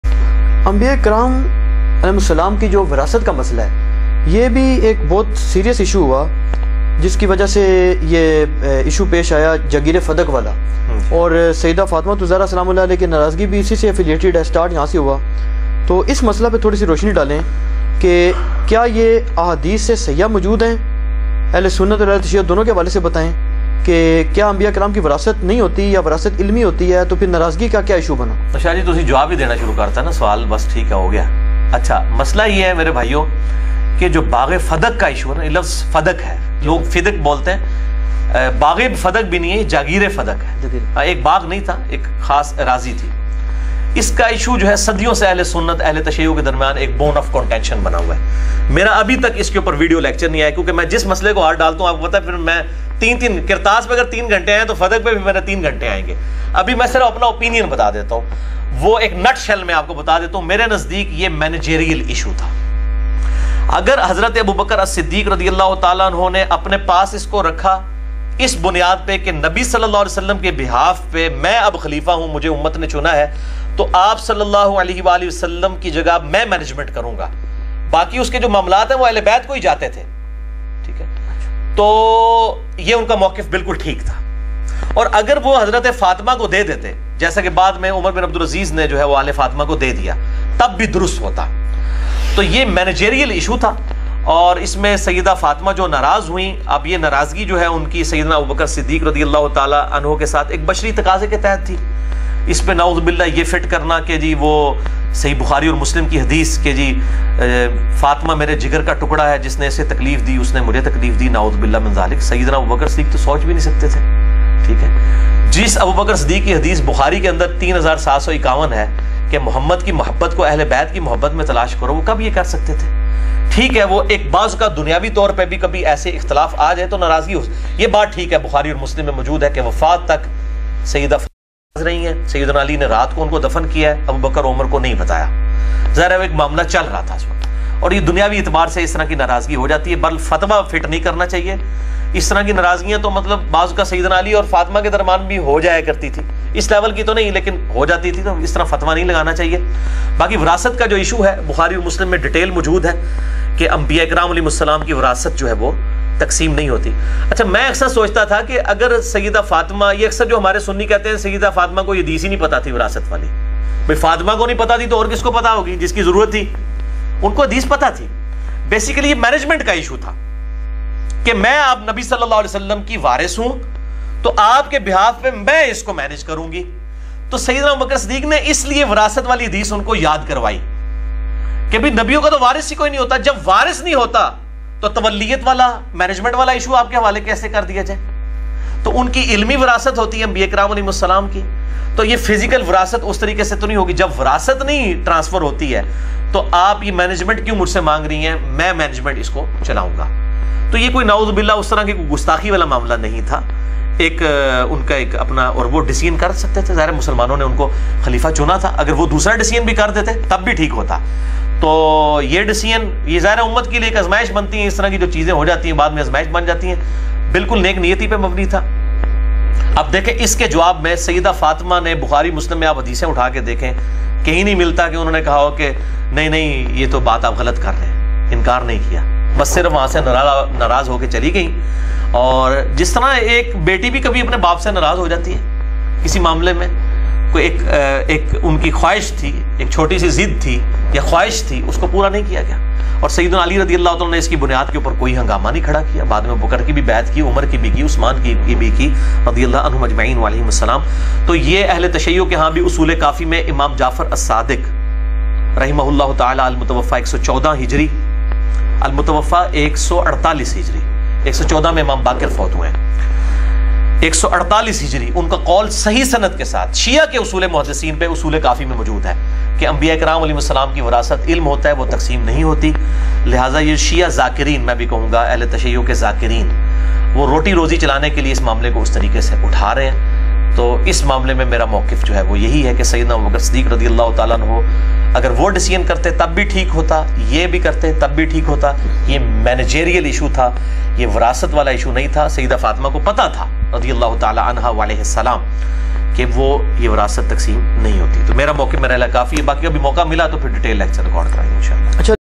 😭 FADAK ki Jageer | BAGH e FADAK | 🔥 Shia Sunni Ikhtelaf | Engineer Muhammad Ali Mirza
Video Clip From 157-a-Mas'alah (Part-1) : 100-Questions on SUNNI & SHIAH Issues with Engineer Muhammad Ali Mirza